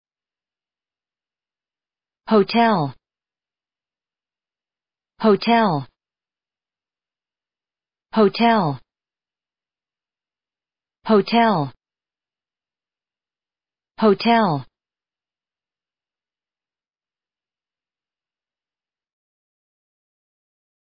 How to pronounce hotel
hotel American English pronunciation. How to pronounce hotel correctly. How to say hotel in proper American English.